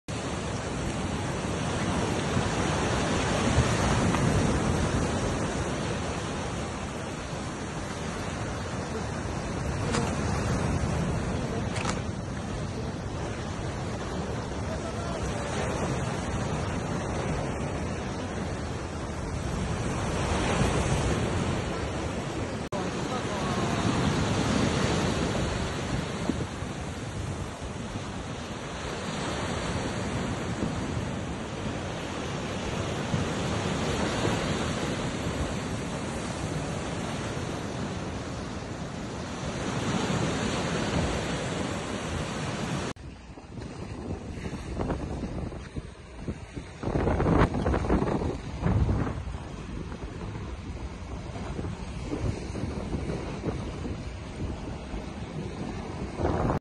Migranti, naufragio a Crotone: il barcone a pezzi sulla spiaggia
CUTRO :: 26/02/2023 ::  La Guardia Costiera ha diffuso le immagini dell'imbarcazione a pezzi ritrovata sulla spiaggia di Steccato di Cutro, prima ancora del sorgere del sole.